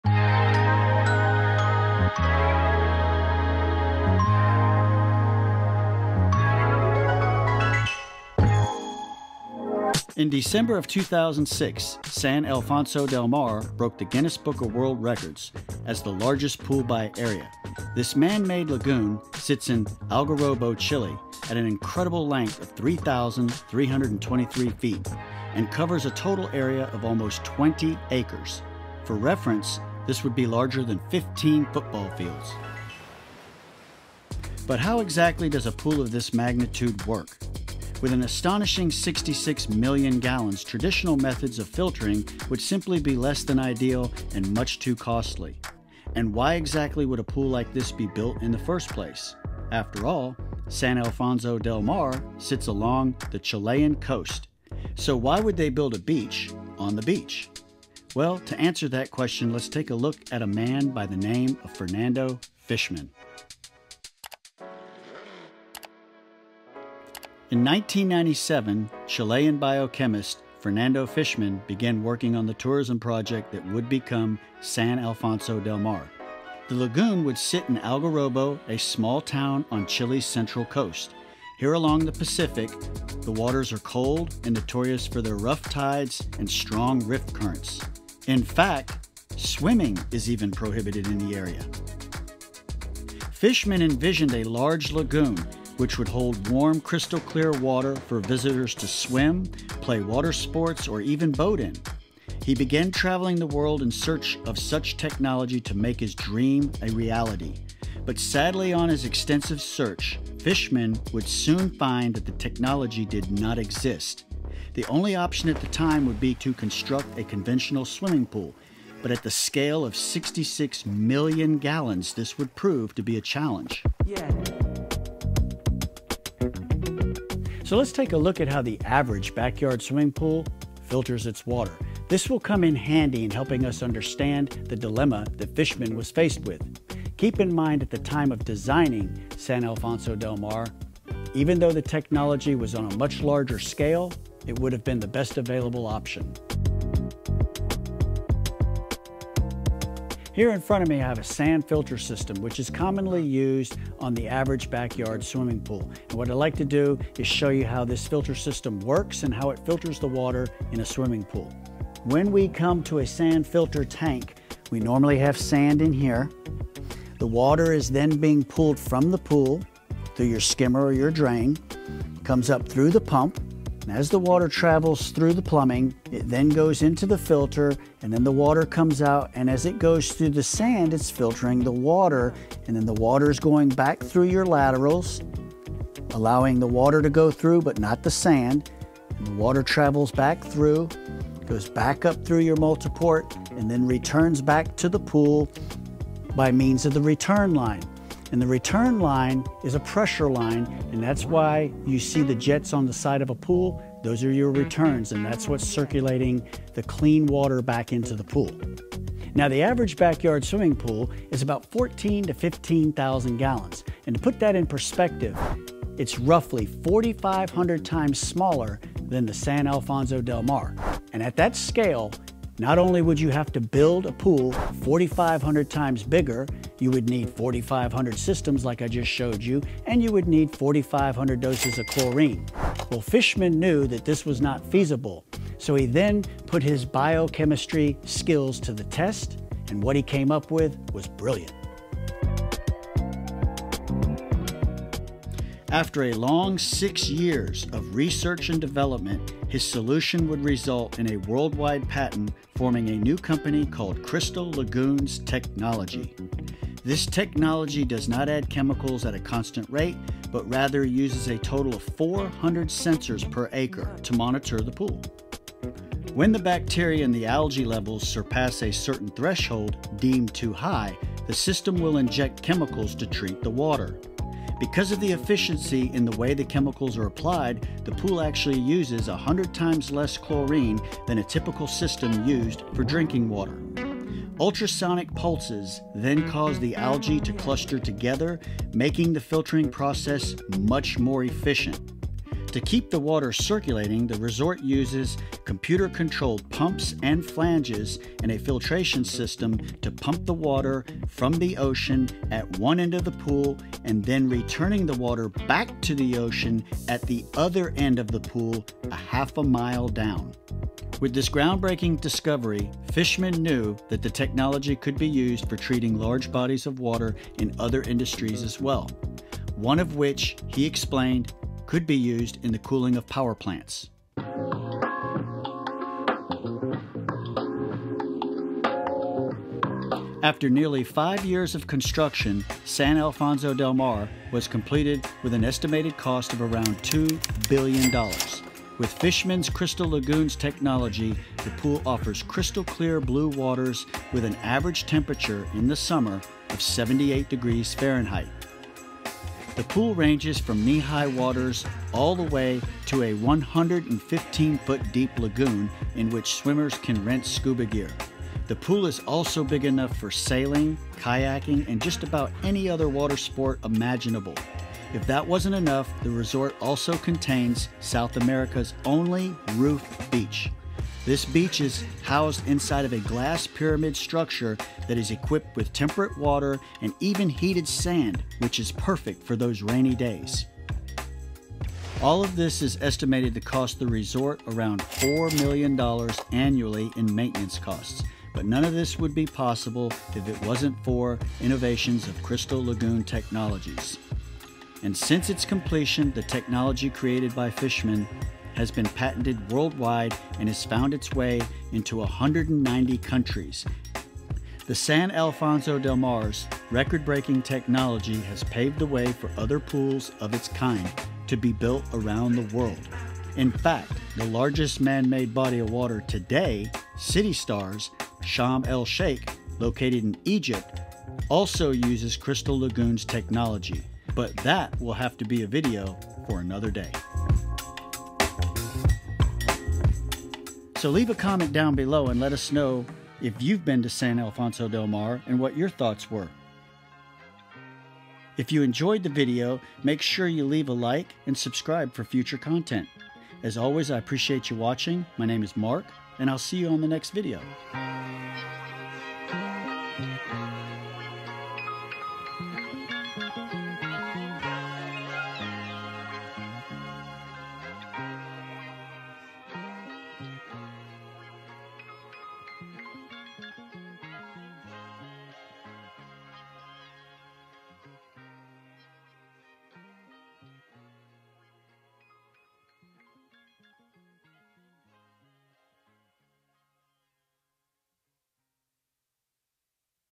How Does This $2 Billion Dollar Swimming Pool Work?
In December of 2006, San Alfonso Del Mar broke the Guinness Book of World Records as the largest pool by area. This man-made lagoon sits in Algarrobo, Chile. Today we look at how a pool of this magnitude operates, how much it costs to run, and much more!

⏩ HAVE A QUESTION FOR MARK? ⏪

for all your pool supplies, and needs

Video Timestamps
00:00 - Intro
00:59 - Fernando Fischmann
02:04 - The Challenge
04:02 - Fischmann's Solution
05:24 - The Final Product